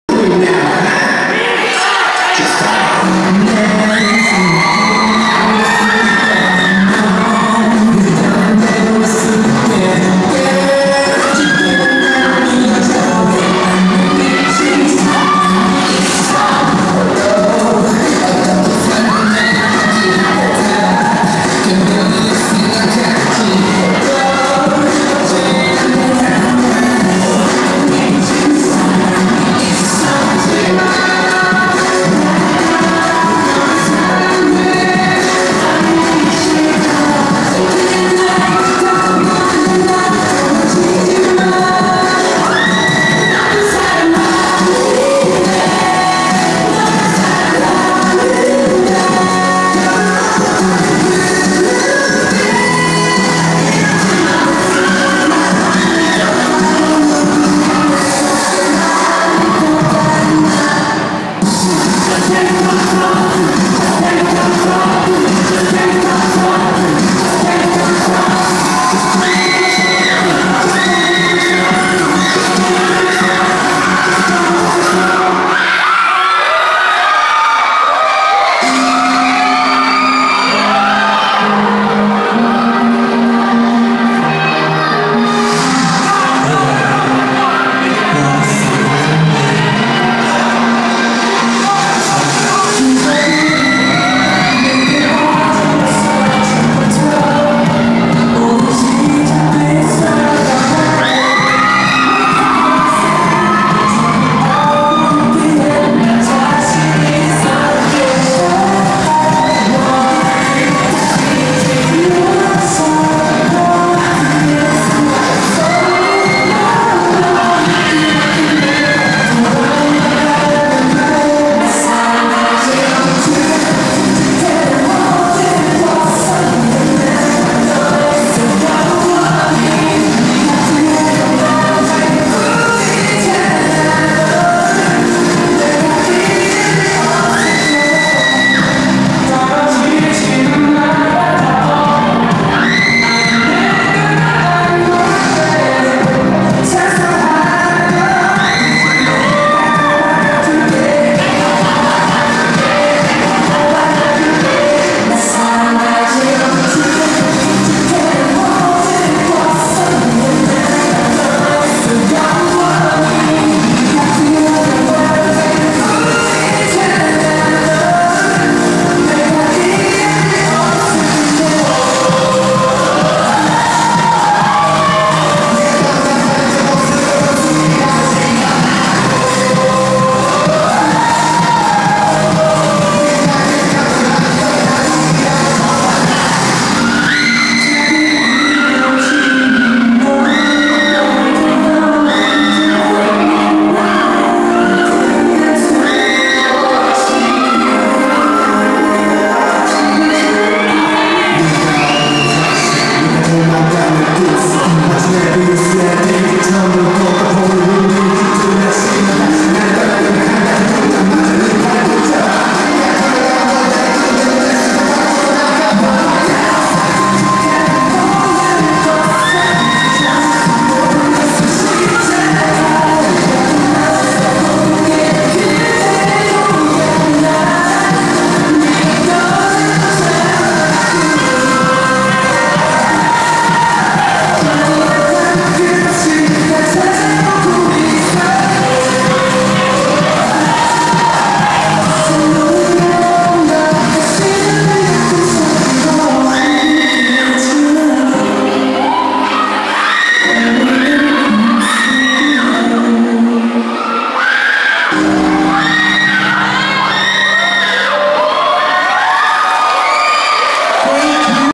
Stardust (VIXX) on Wassup!: KPop Wave
[07-06-14]
@ San Juan Gym Function Hall